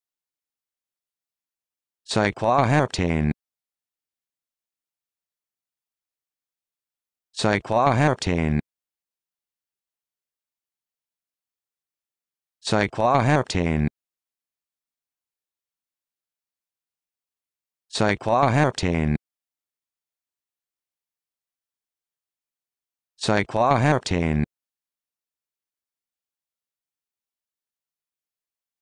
How to Pronounce Cycloheptane
This video shows you how to pronounce Cycloheptane